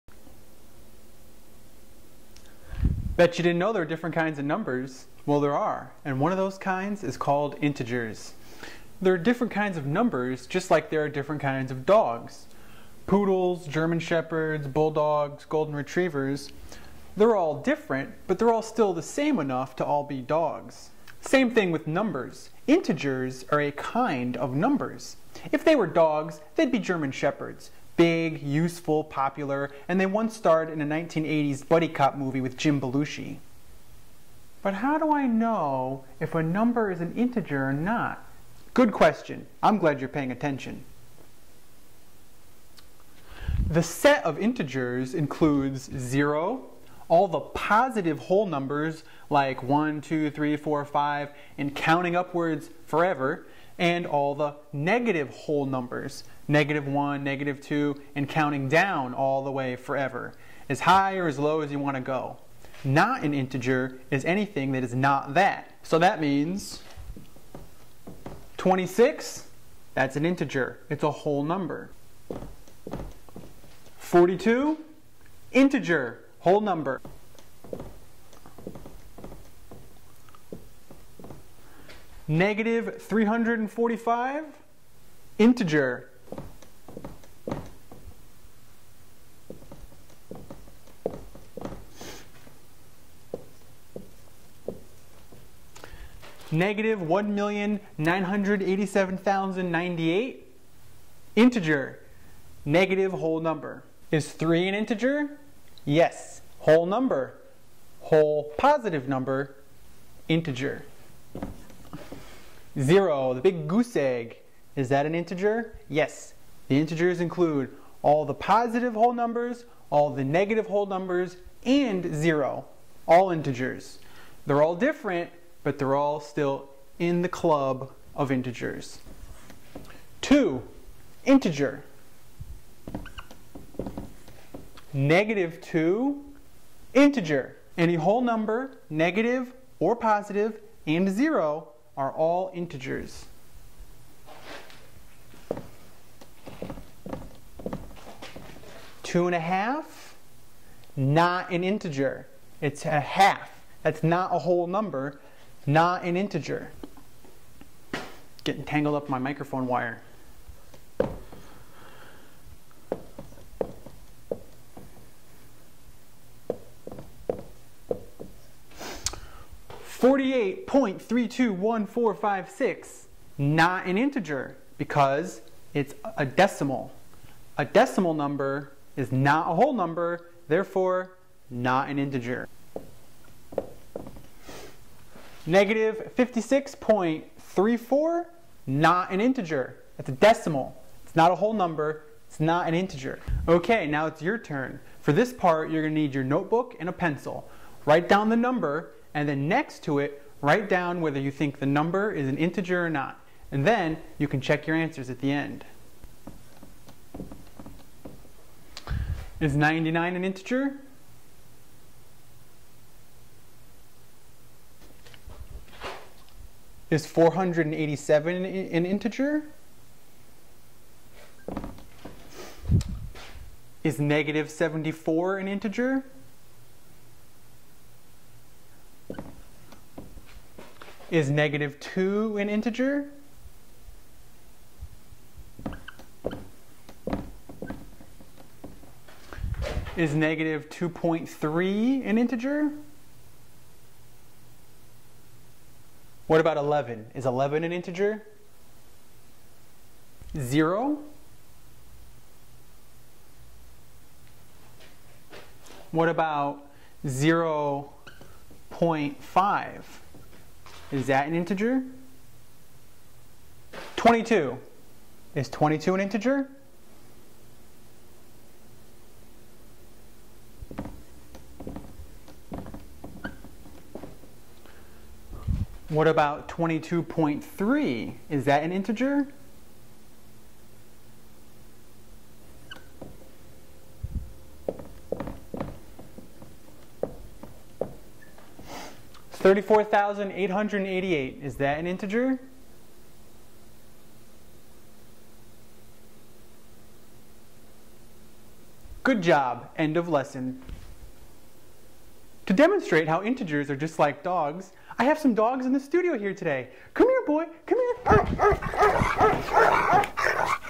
What are Integers?
Scott explains what are integers.
This video is a free sample from Homestead Homeschool's Advanced Curriculum.